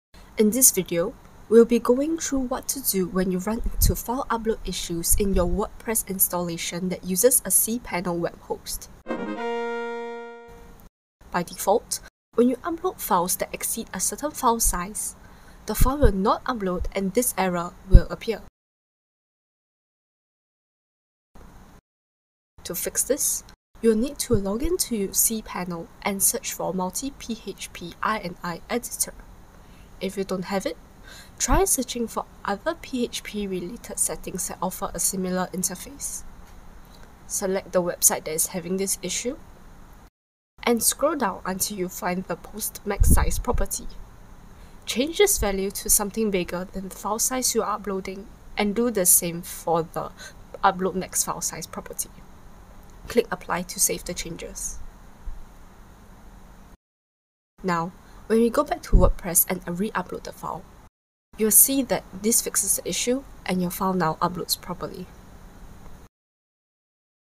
How to fix file upload issues in Wordpress for cPanel web hosts
If you are running into issues with uploading your files into your WordPress website, here's how to fix it.

0:00 Introduction
0:22 The fix